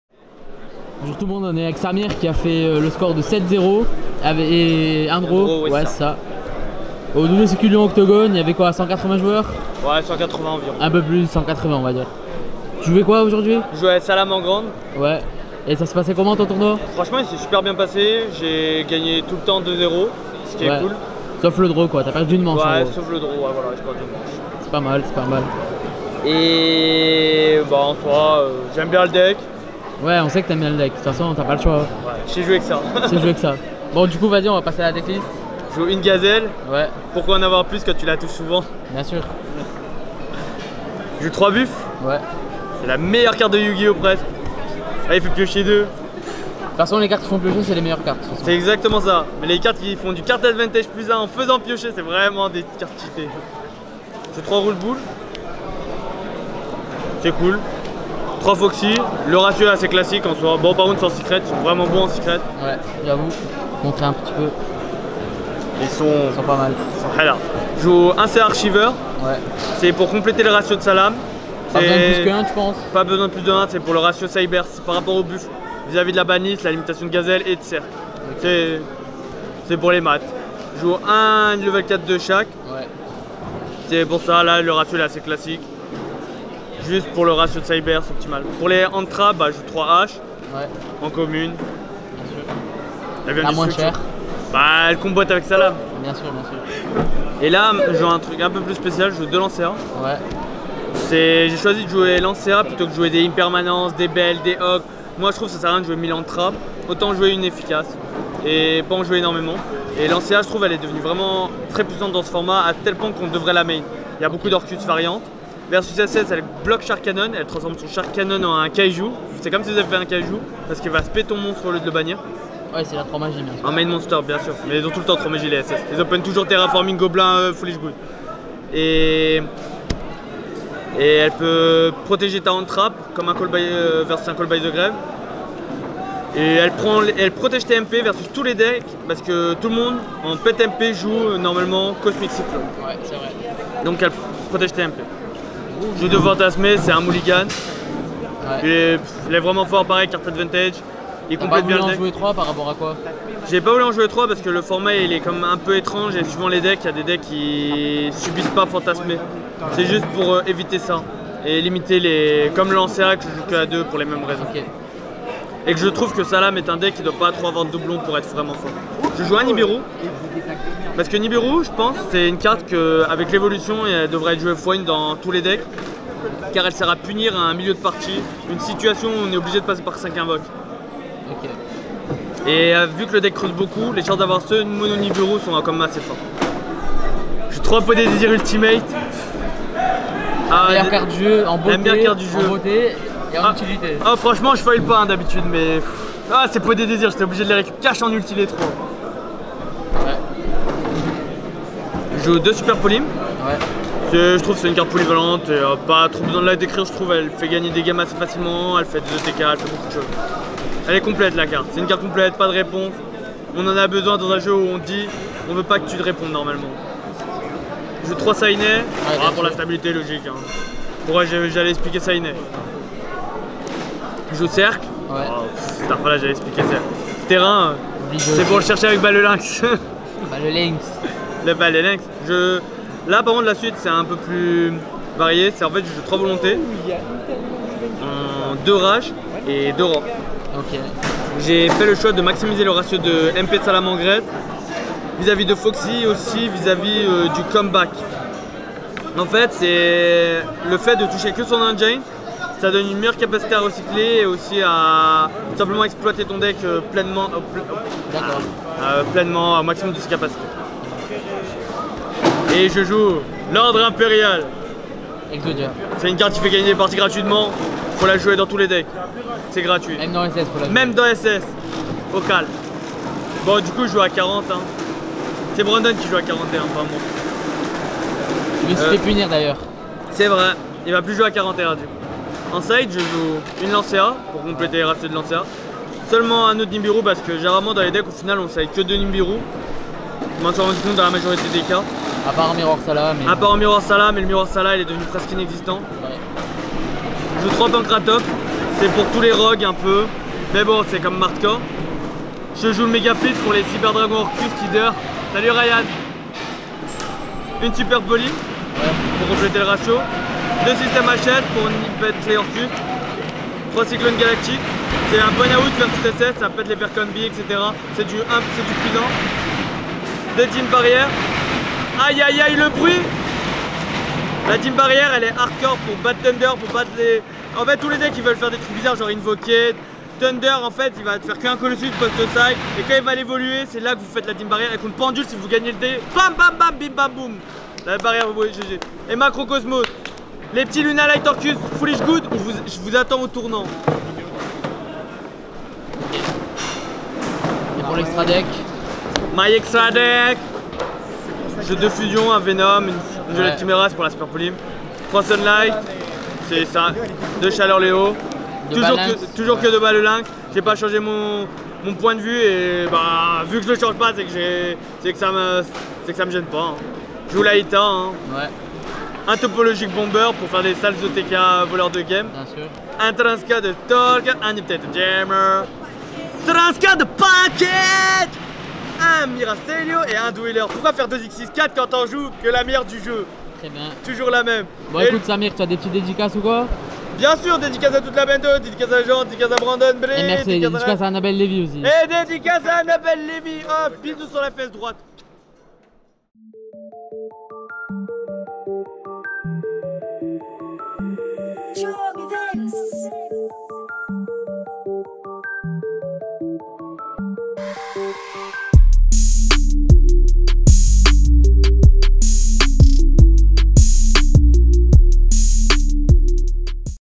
WCQ Lyon 2019 : 1 ère place Samir Bachar - Salamangreat
Bonjour tout le monde, on se retrouve aujourd’hui pour le deckprofile de Samir qui remporte le WCQ de Lyon (180+ joueurs) avec salamangreat !